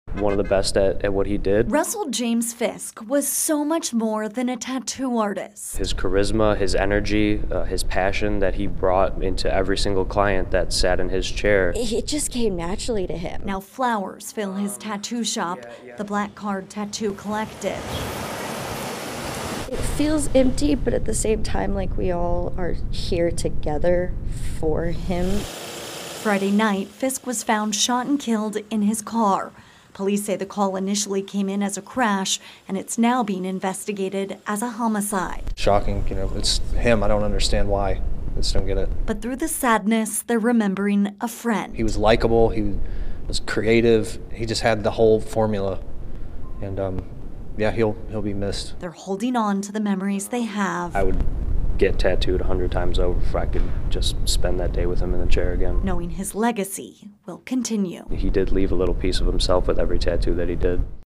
Well-known tattoo shop owner gunned down in Northland
A well-known tattoo artist was gunned Friday night in his car at Barry Road and North Marston Avenue. Russell James Fisk, 30, owned the Black Card Tattoo Collective.